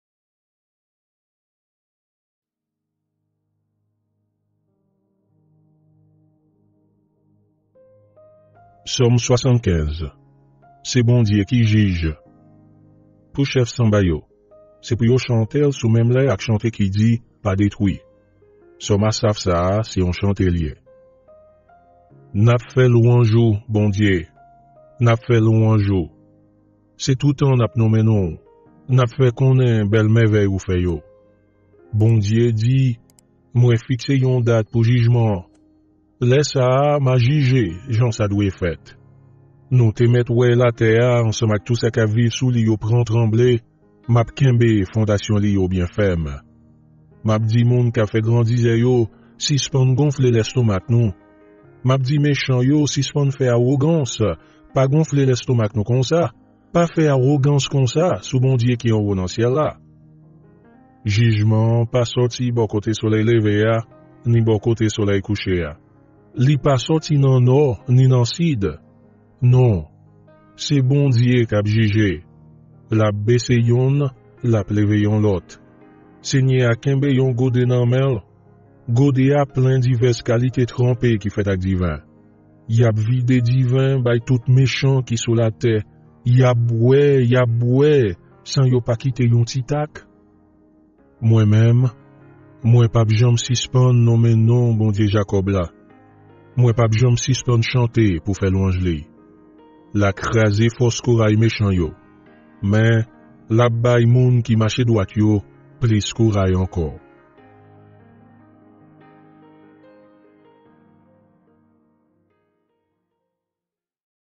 Som 75 Ak Mizik - Bib La Kreyol Ayisyen | Psalm 75 With Music - The Bible Haitian Creole
Liv Sòm la se yon seri 150 chante, lapriyè ak lòt zèv ki fòme 19yèm liv Ansyen Testaman an. Tradisyonèlman bay wa David kredi, Sòm yo te yon pati entegral nan adorasyon pou plizyè milenè, tankou kantik, litiji, ak lapriyè pèsonèl.

The book of Psalm is an assortment of 150 songs, prayers, and other works that make up the Old Testament's 19th book. Traditionally credited to King David, the Psalms have been an integral part of worship for millennia, such as hymns, liturgy, and personal prayer.